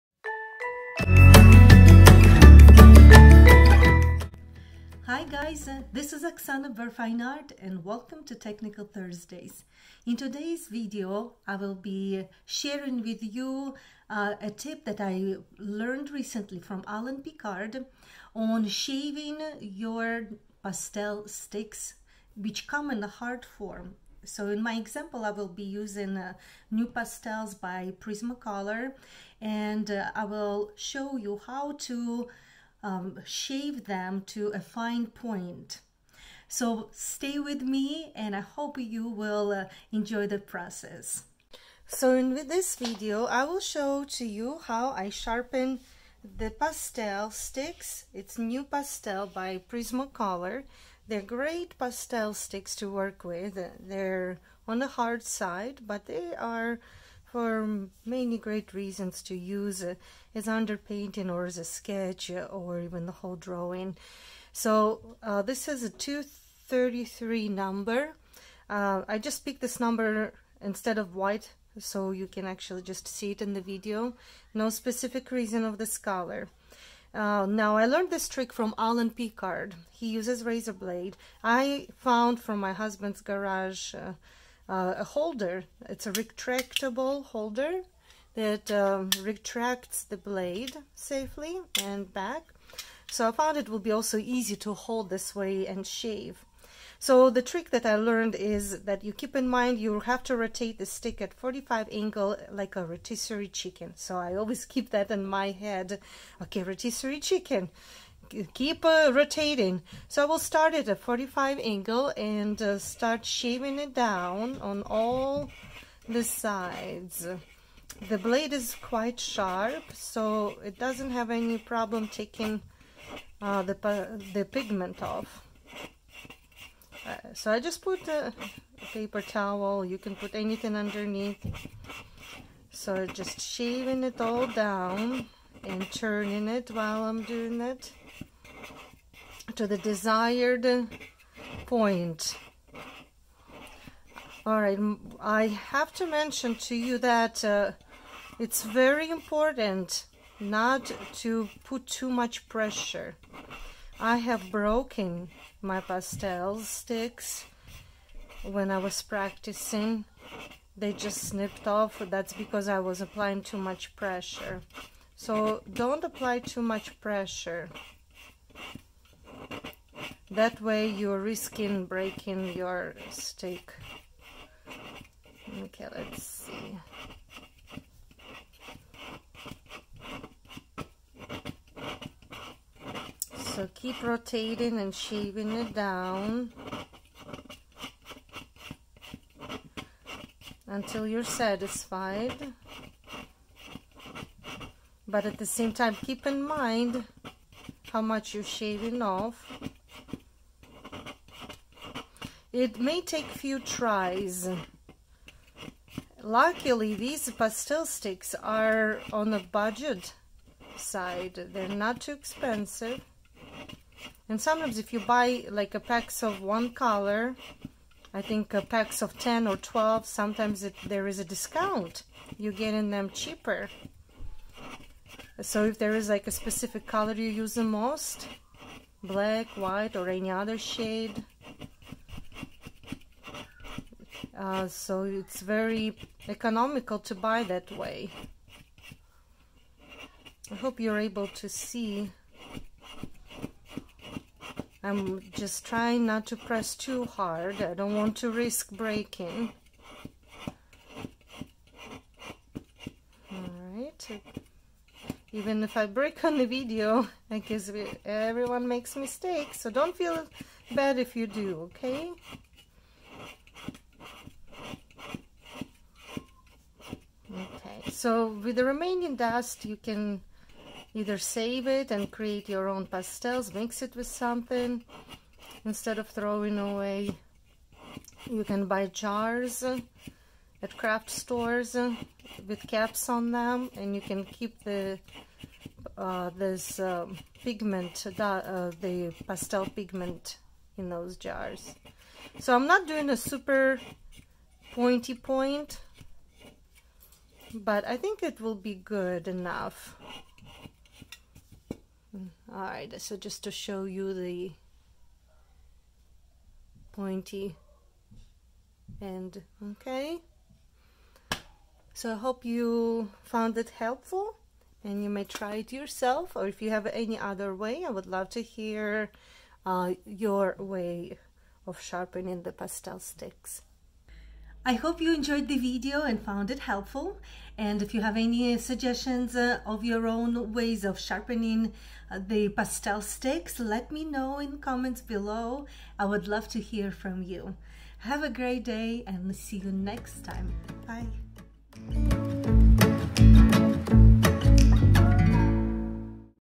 Sharpening Pastels | Art Tips
In this video I’m sharing with you a tip on how to sharpen your pastels, but specifically in this case hard pastel sticks like Nupastels by Prismacolor.  I originally learned this tip from a pastel artist I admire, Alain Picard, and I have been using it in my own drawing practice since then.
You can use this trick on other brands of hard pastel sticks as well, so it’s up to you what you use for your own practice.
I have personally tried sharpening pastels in many ways before.  I tried brass and steel manual sharpeners, peelers, exacto knives, and other methods I have forgotten about.  But this rather simple and low cost method attracted me because of very sharp blades that are very cheap to replace and just its simplicity.  I don’t mind that it takes time to sharpen but once you get it to the point from pastel stick’s original state, it would become easier to re-sharpen in my opinion.
You may or may not find this method working for you but my job is to share with you what works for me and hopefully it will be a revelation for others.